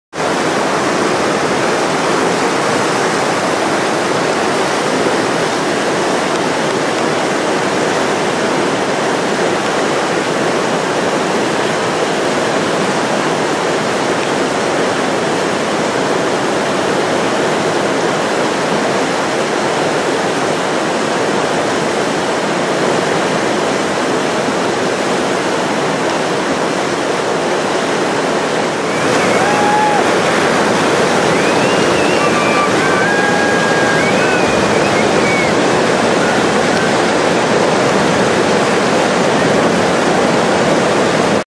First Descents in Action
The Middle Fork of the Flathead head river was a constant test of stability. Choppy waters, freezing temps and tight bends going right left right for seven miles.